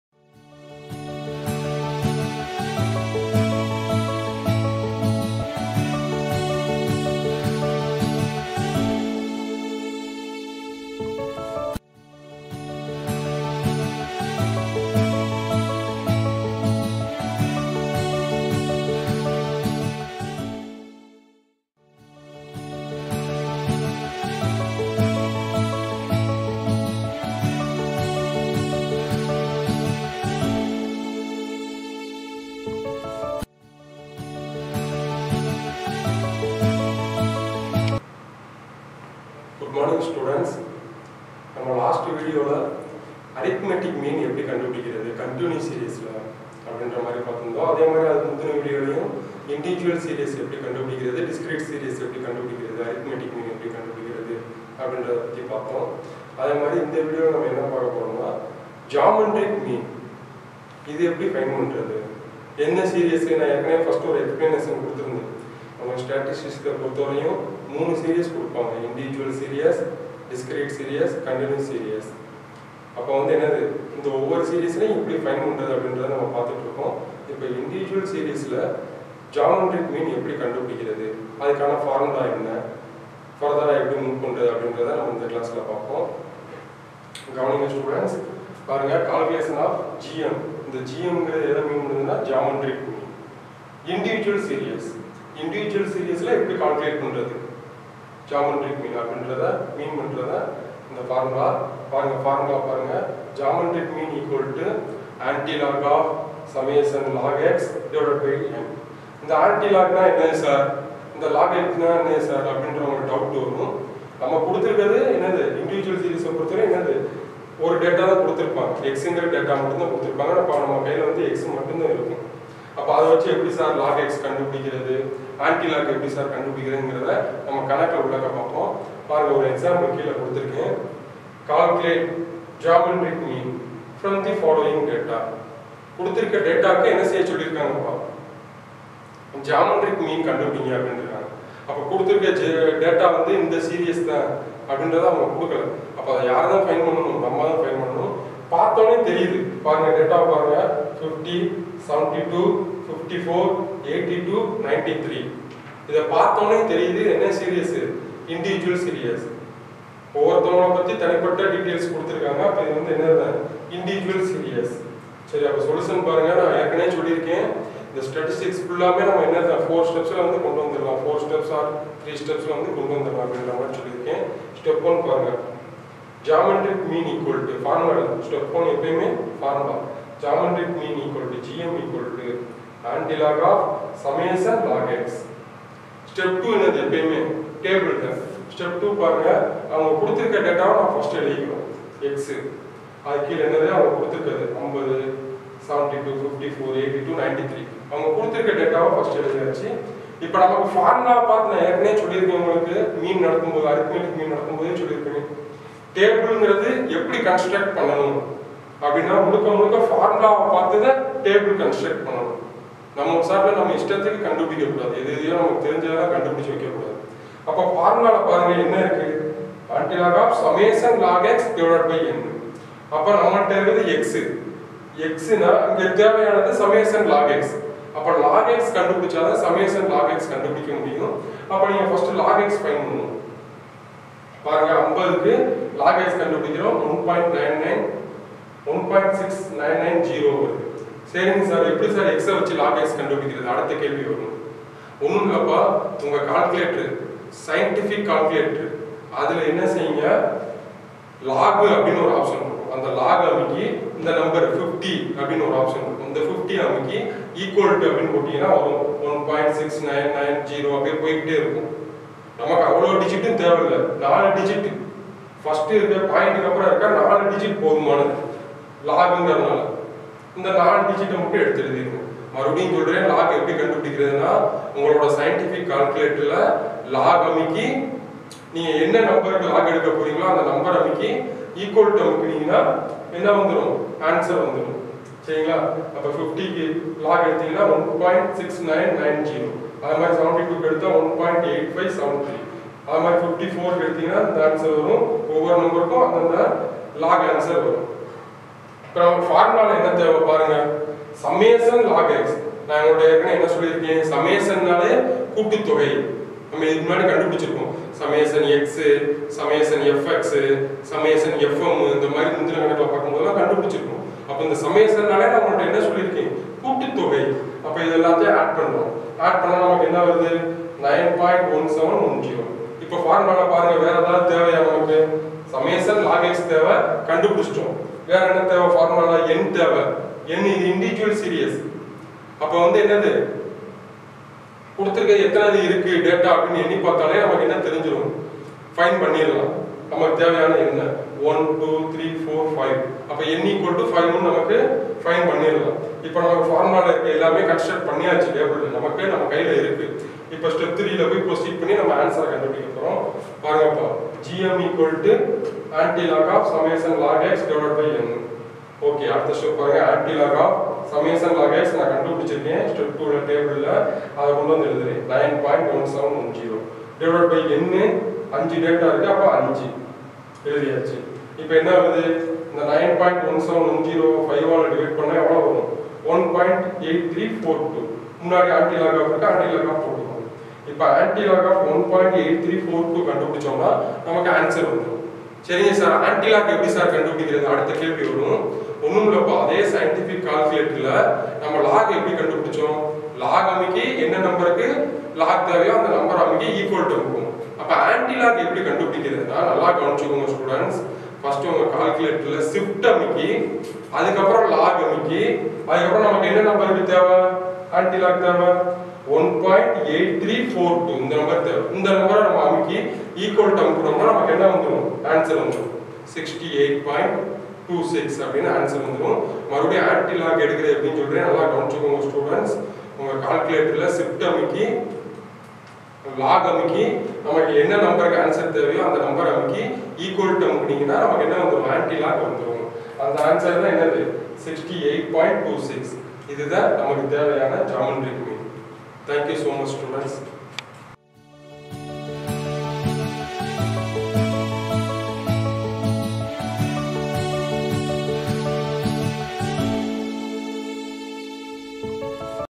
Geometric Mean | Individual Series | V.H.N.S.N.C (Autonomous)
A simple explanation about Geometric Mean.

Mr.M.Mohamed Yazer Arabath M.Sc., M.Phil., PGDCA
Assistant Professor in Mathematics (SF)
VHNSN College (Autonomous)
Virudhunagar - 626001, Tamil Nadu, India.